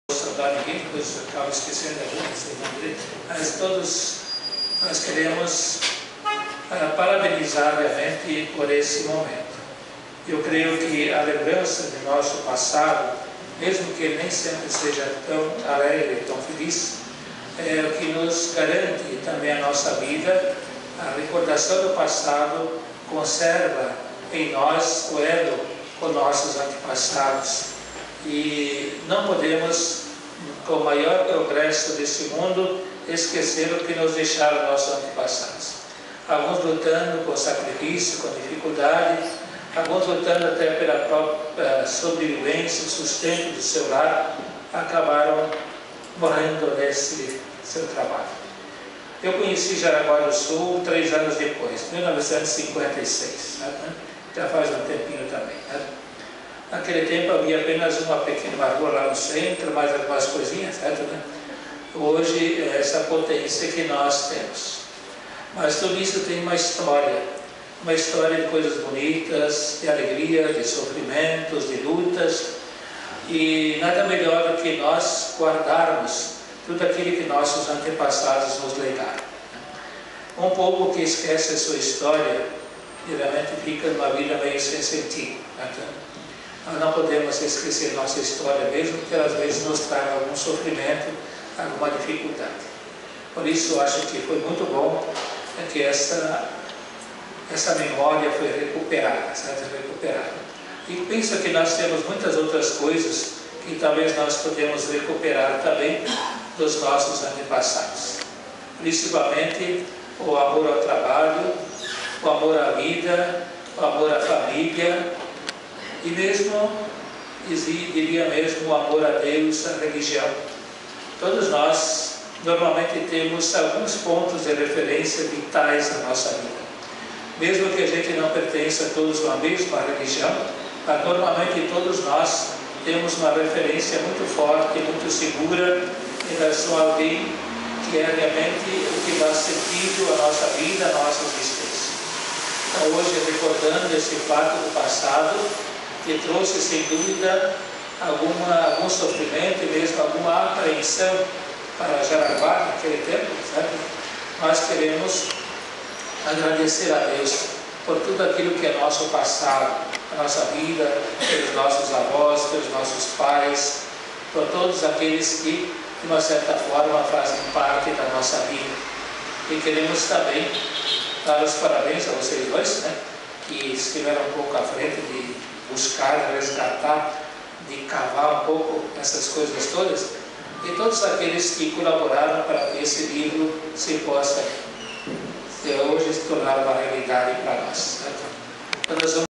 Explosão Fábrica de Pólvora - Lançamento de Livro - 02
Jaraguá do Sul [SC], Explosão Fábrica de Pólvora - Lançamento de Livro - 02.
Padre Francisco Schen, gravação pelo historiador Felipe Corte, do Arquivo Histórico "Pastor Wilhem Lange" de  Guaramirim [SC].
Ademir Pfiffer - Historaidor